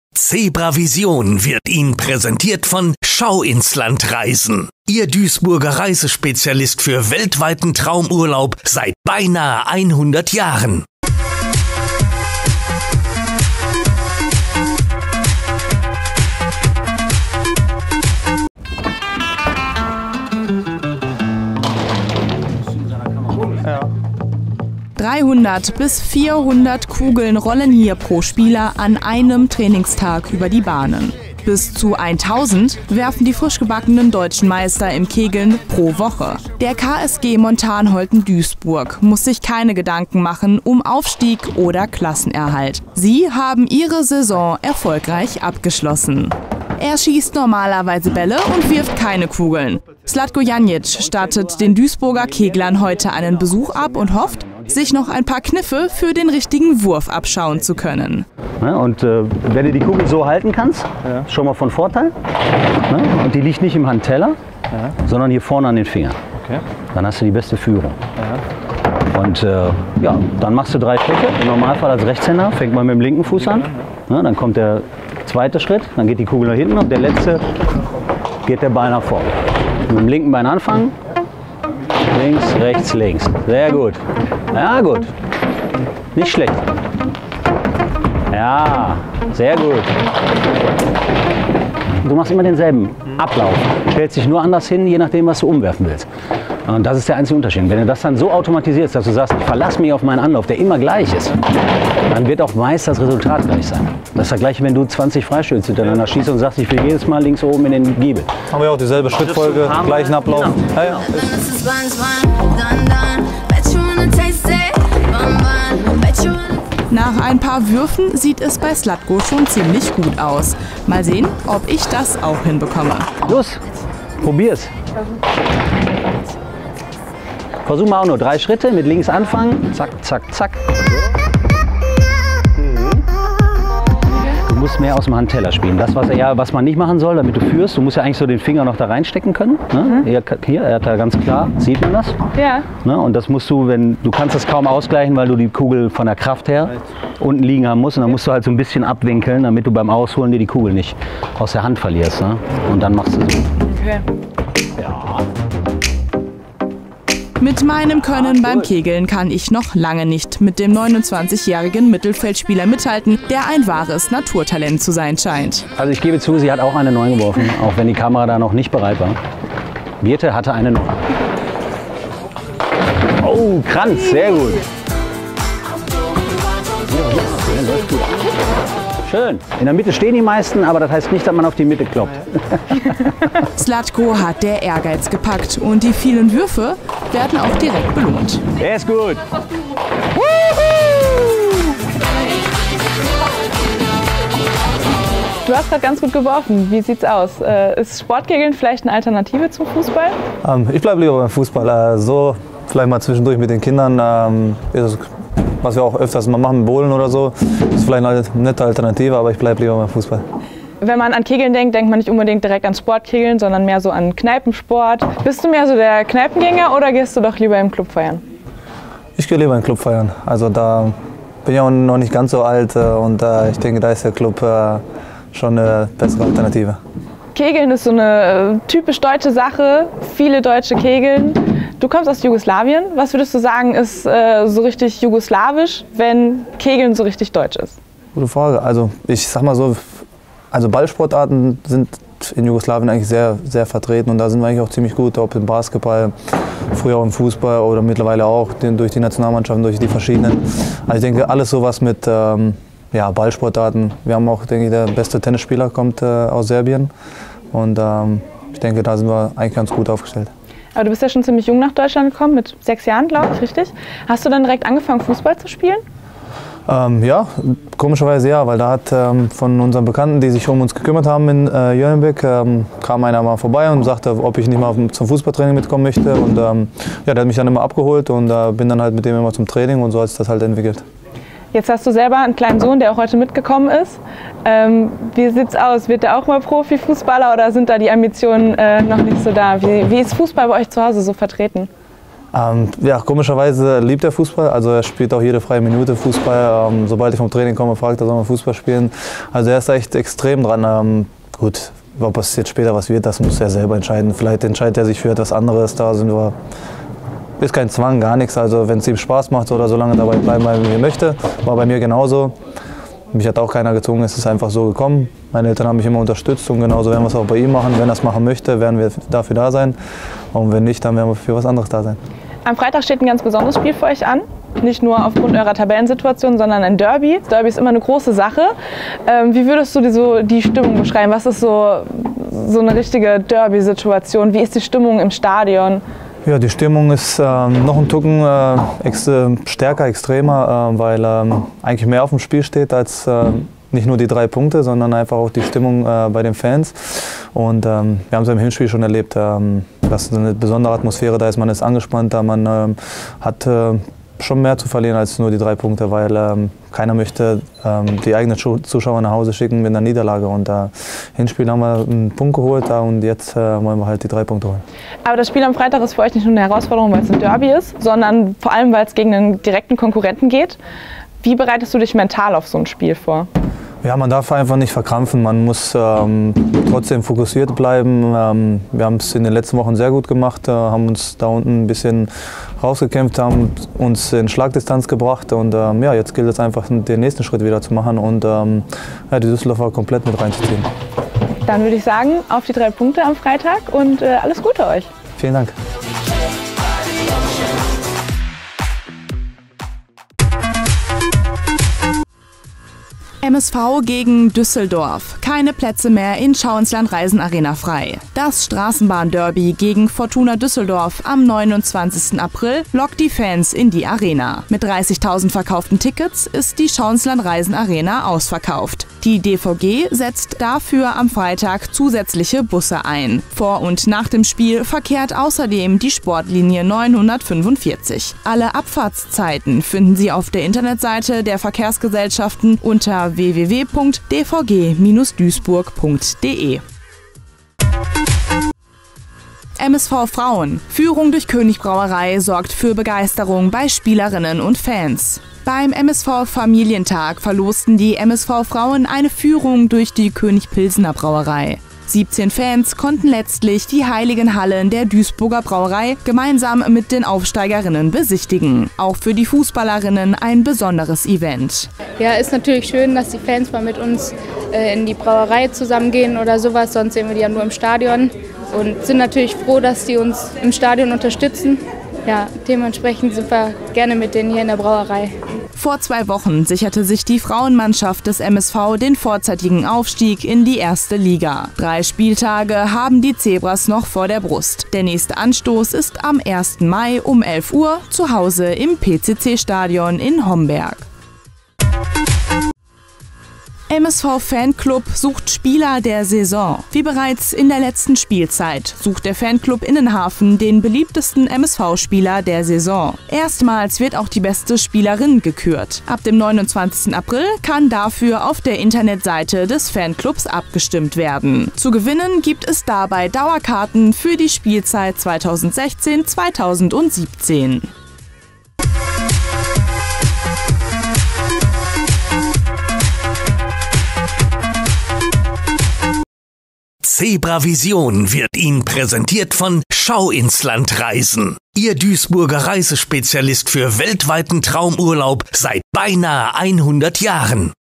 ZebraVision - #42 – 2015/2016 – Thema: Spielerporträt Zlatko Janjic
ZebraVision – das MSV-Fanmagazin vom 28. April 2016. Das Thema: Normalerweise steht Zlatko Janjic auf dem Fußballplatz – heute holen wir ihn einmal aus seiner Komfortzone und gehen mit dem gebürtigen Jugoslawen kegeln. +++ Keine Karten mehr für Derby gegen Düsseldorf +++ MSV Frauen besuchen Köpi-Brauerei +++ Fanclub Innenhafen sucht Spieler und Spielerin der Saison